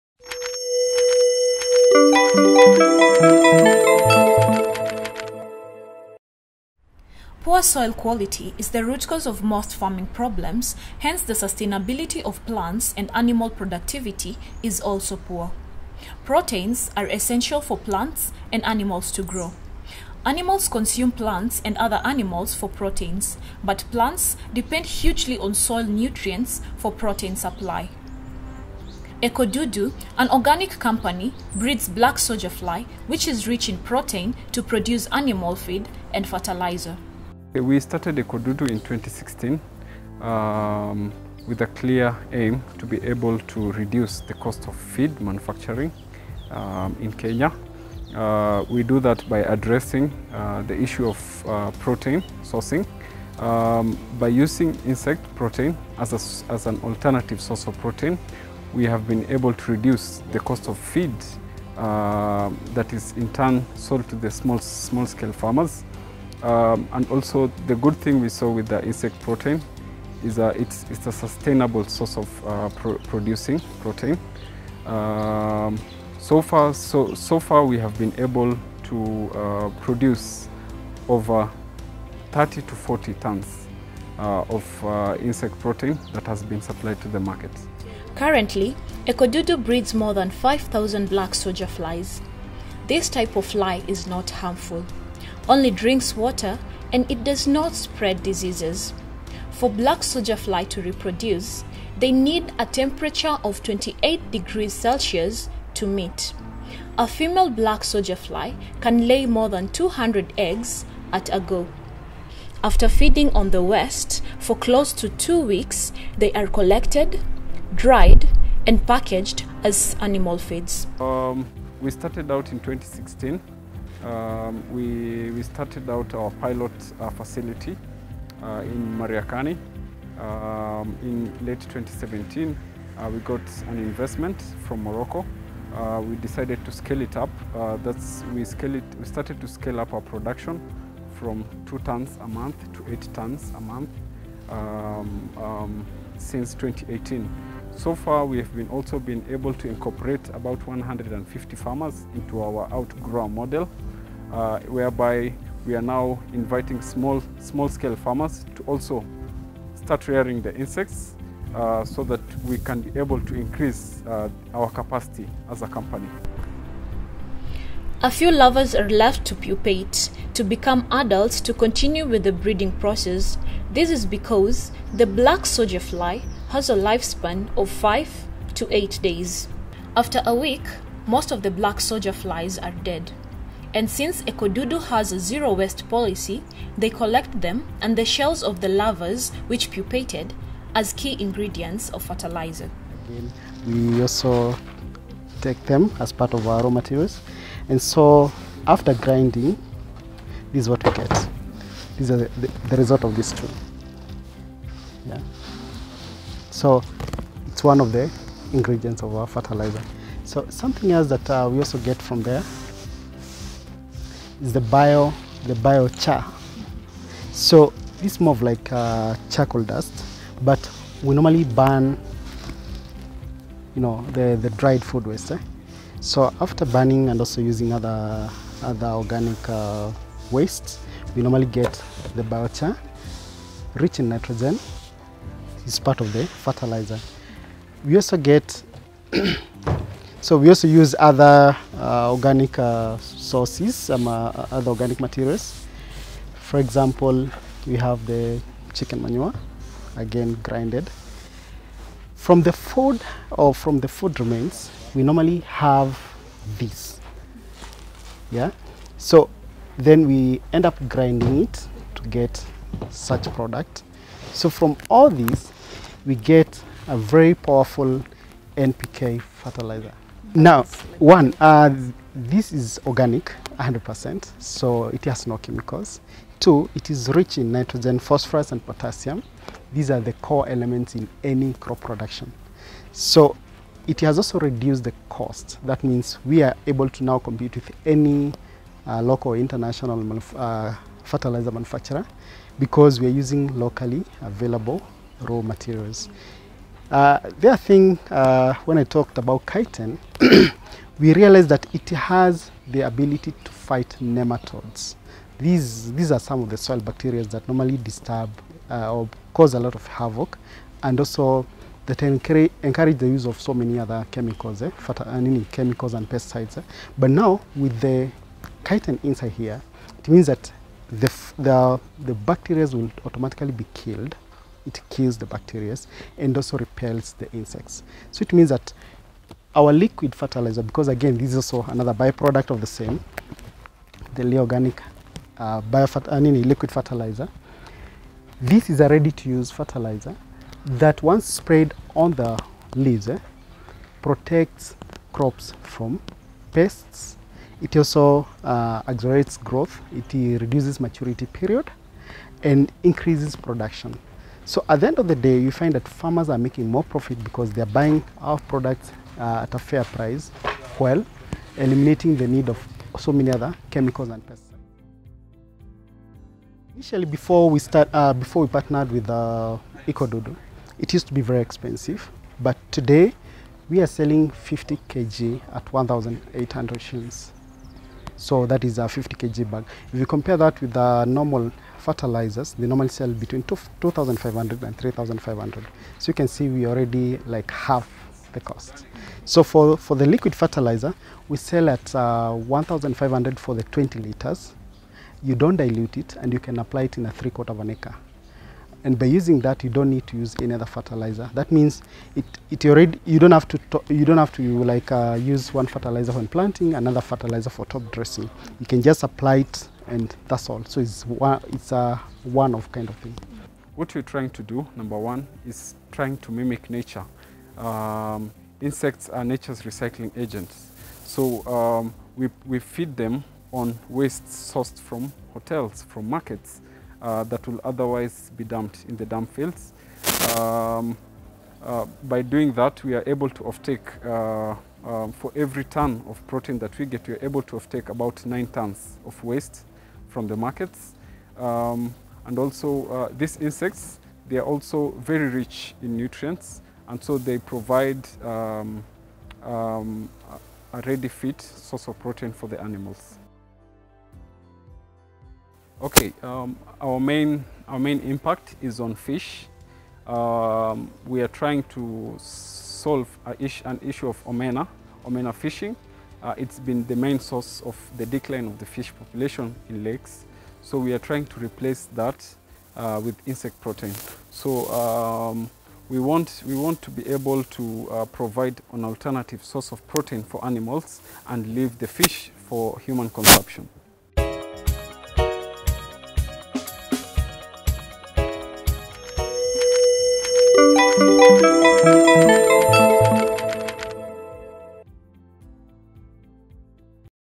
Ecodudu: Insects are the future. 🐜
This is feature on Ecodudu a sustainable company that provides an alternative solution to protein supplements for animals using Blacksoldier flies.Ecodudu, an organic company, breeds black soldier fly which is reach in protein to produce animal feed and fertilizer.